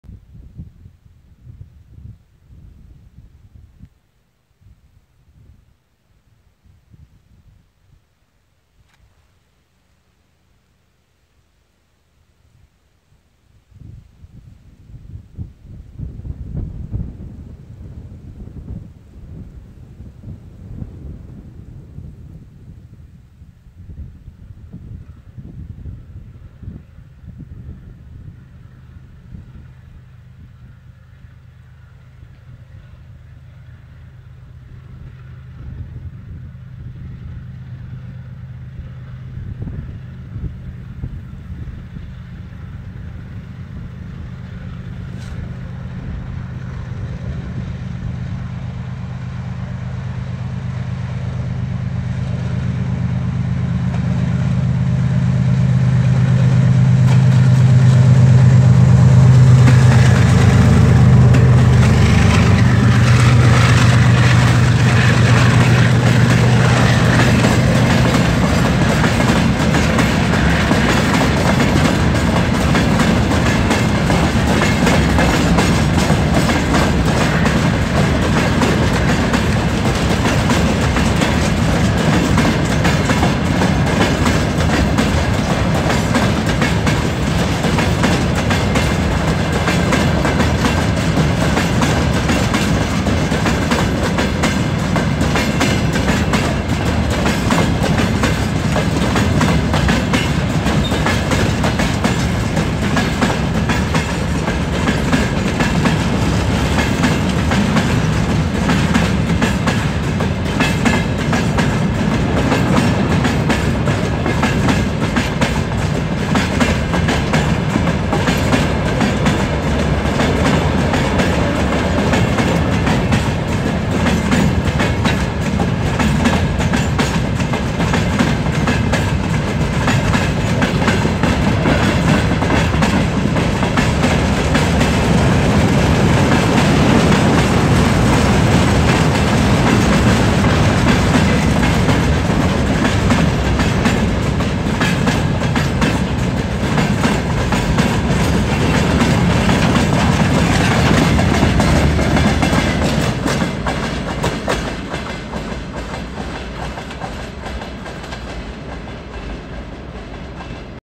Съемка поезда с дерева на перегоне Чадыр-Лунга - Абаклия [CFM]
ТЭ33АС-3007 |  TE33AC-3007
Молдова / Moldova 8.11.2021
Порожний поезд
Перегон: Чадыр-Лунга - Абаклия